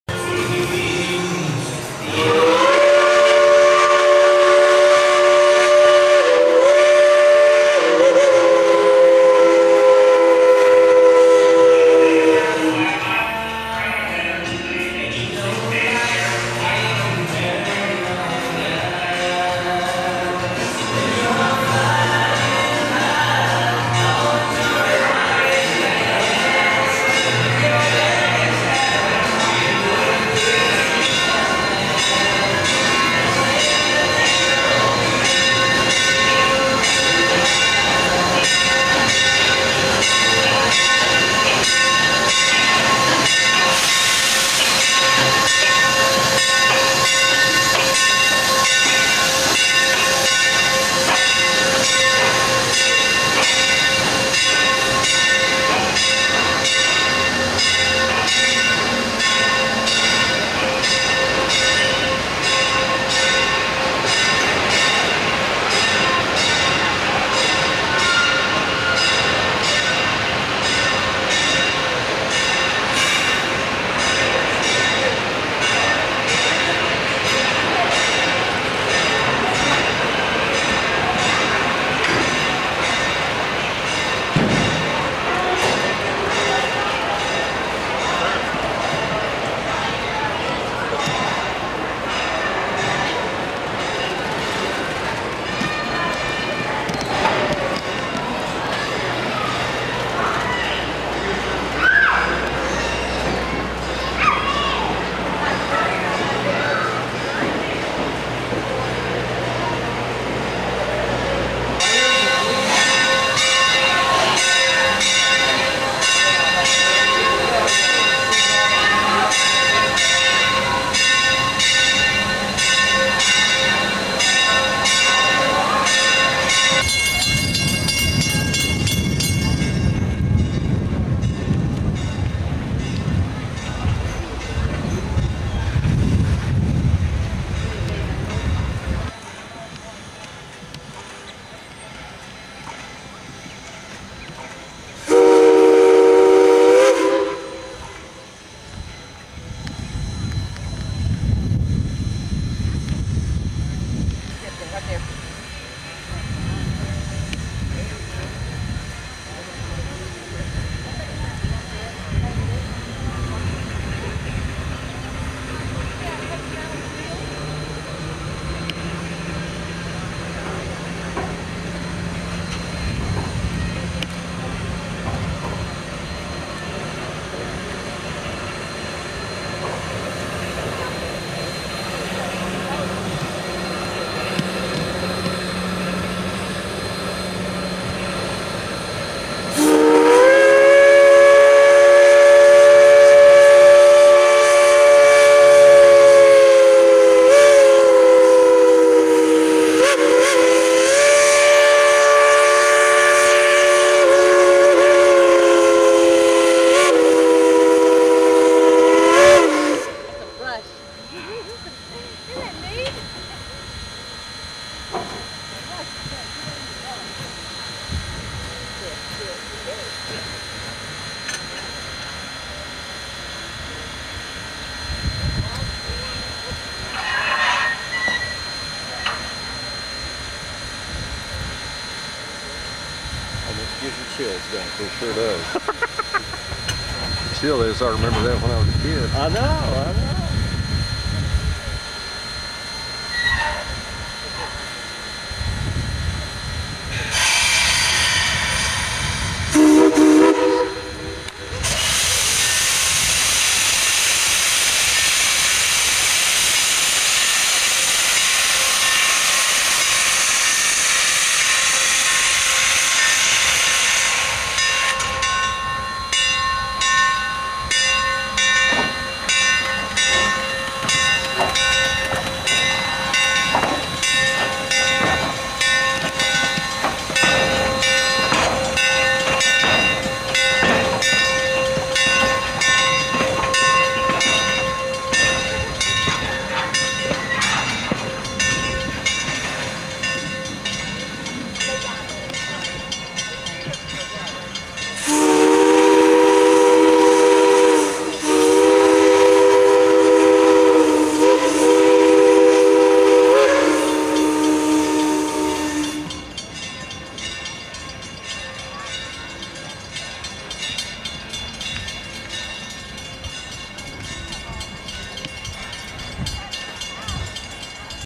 Excursion steam engine reverses direction on turntstile
4-6-0 steam engine of the Tarantula line enters historic Fort Worth Stock Yards and is turned around on a turnstile.  Circa 1995.  Video by yours truly.